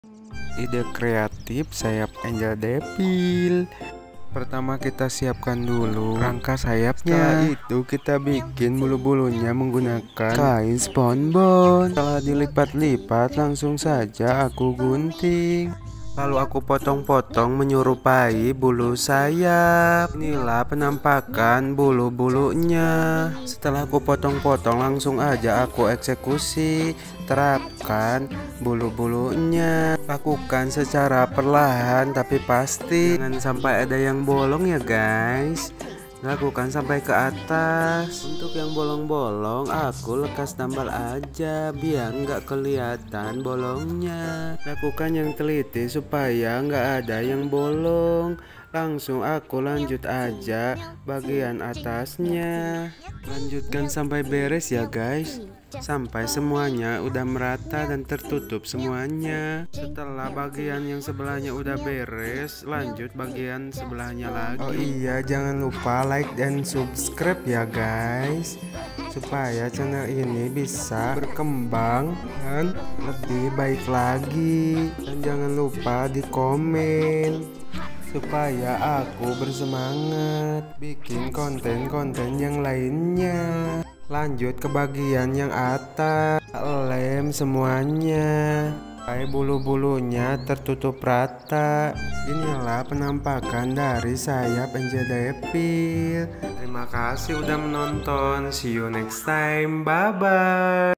Cara membuat sayap malaikat [ Angel Devil ] Versi Merah
Cara membuat sayap malaikat [ Angel Devil ] Versi Merah | Chainsaw man | Cosplay Majalengka

DIY Sayap Malaikat dari Kain sponboand dan kawat